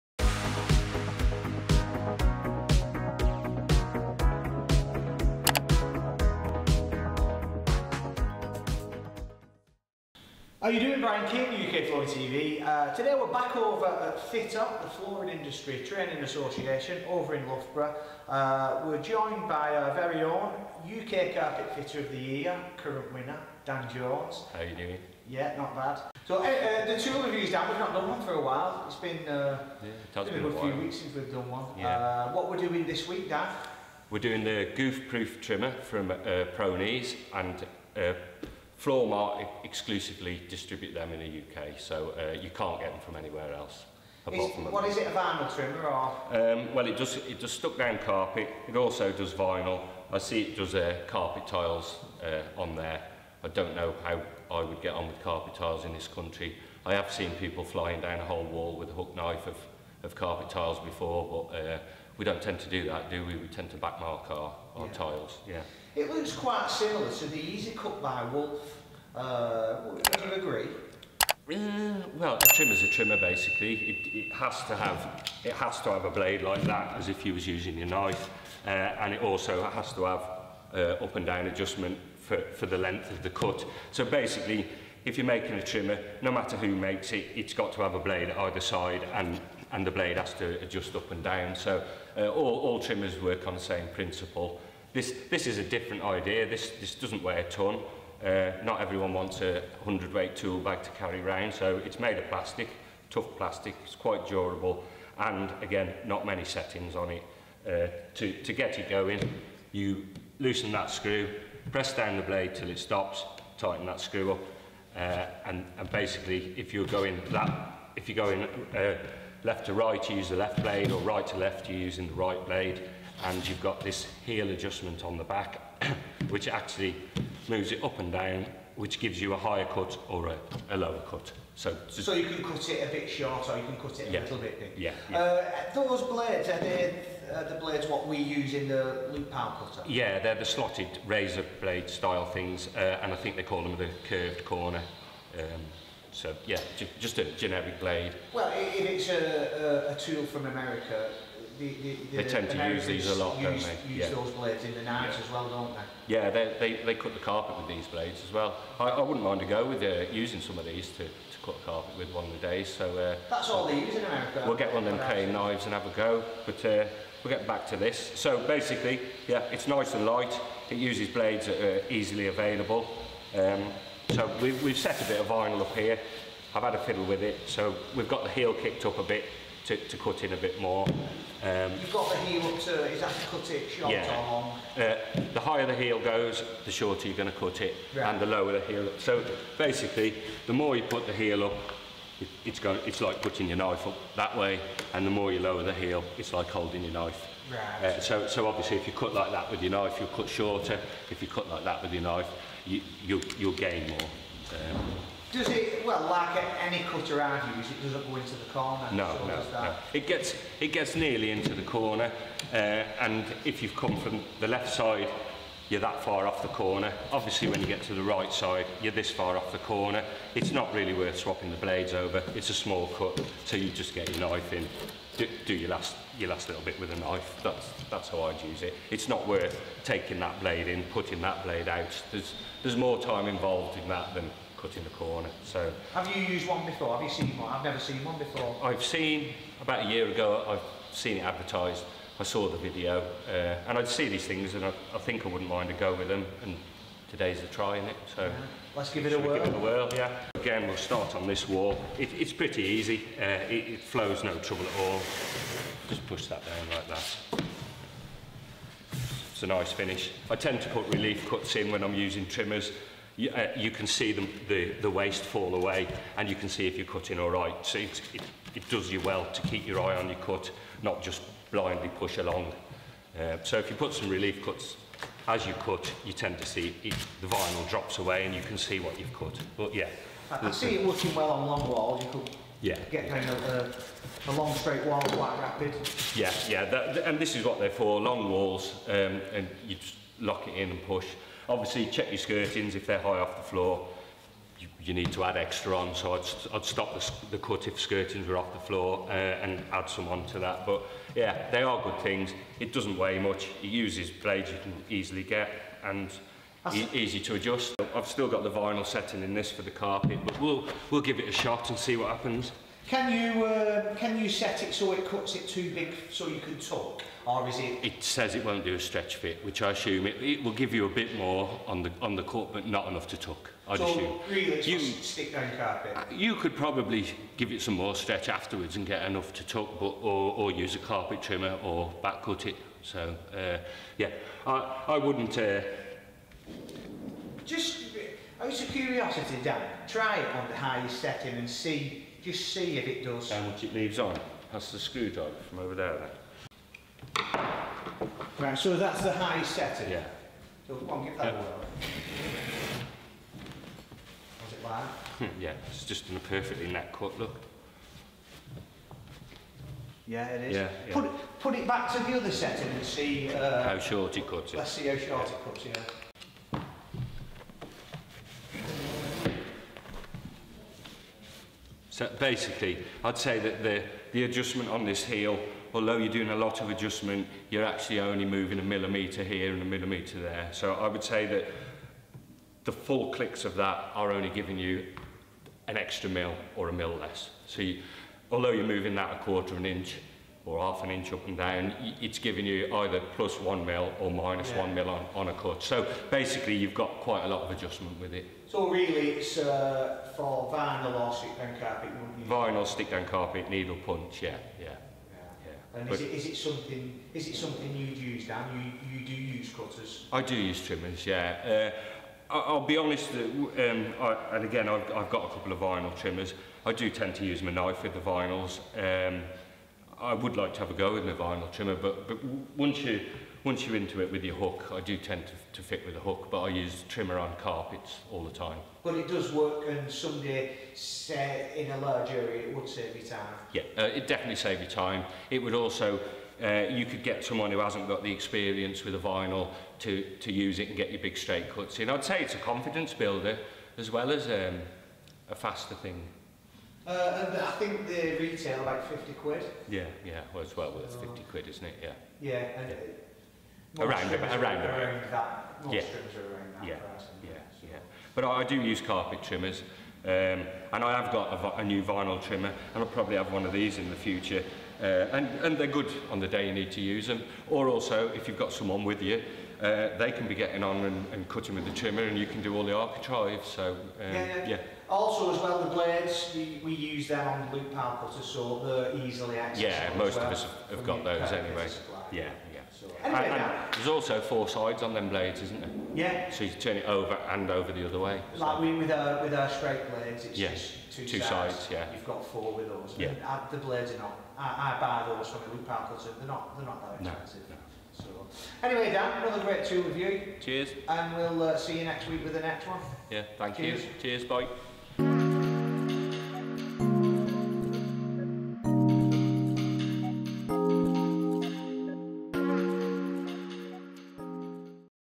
ProKnee Goof Proof Review With UK Flooring TV
Welcome to our first YouTube video. Today we reviewed ProKnee's very own Goof Proof Wall Trimmer with UK Flooring TV. Brian King and Dan Jones had nothing but appraisal for this time saving product.